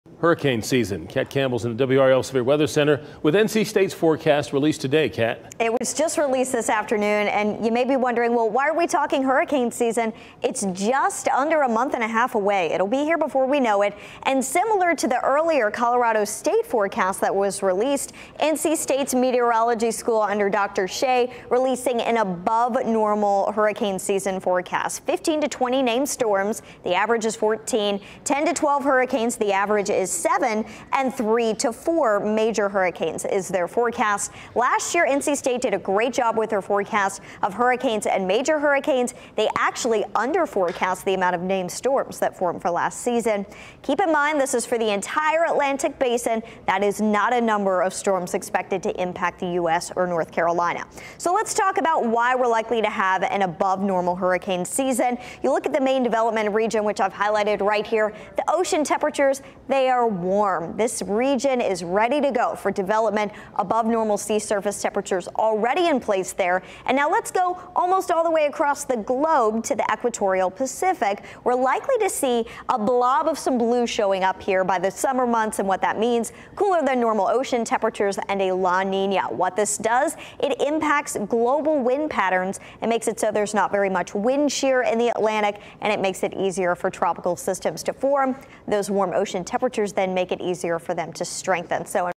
NC State researchers predict active Atlantic hurricane season with 15-20 named storms
The 2024 Atlantic hurricane season will see 15 to 20 named storms forming in the Atlantic basin, according to researchers at North Carolina State University. The number of named storms predicted is significantly higher than the long-term average, and moderately higher than recent 30-year averages, according to Lian Xie, professor of marine, earth and atmospheric sciences at NC State. NC State researchers predict:

- 15 to 20 named storms
- 10 to 12 may grow strong enough to become hurricanes (the historical average is six)
- Three to four becoming major hurricanes

The long-term (1951 to 2023) average of named storms is 11, and the more recent average (1994 to 2023) is 14 named storms. The Atlantic hurricane season runs from June 1 through Nov. 30. The Atlantic basin includes the entire Atlantic Ocean, the Gulf of Mexico and the Caribbean Sea. NC State researchers said the Gulf of Mexico will also see a slightly more active hurricane season. Of the 15 to 20 named storms predicted across the entire Atlantic basin, Xie’s data indicates the likelihood of five to seven named storms forming in the region, with two to four of them becoming hurricanes and one to two becoming a major hurricane. Historic averages for the Gulf are three named storms and two hurricanes. Xie’s methodology evaluates more than 100 years of historical data on Atlantic Ocean hurricane positions and intensity, as well as other variables. It includes weather patterns and sea-surface temperatures to predict how many storms will form in each ocean basin.

The forecast is a collaboration between NC State’s departments of marine, earth and atmospheric sciences and computer science.

NC State adjunct assistant professor of marine, earth and atmospheric sciences Xia Sun also contributed to the research.

Earlier this month, Colorado State University researchers also predicted a busy hurricane season. It includes a predicted:

- 23 named storms
- 11 hurricanes
- Five major hurricanes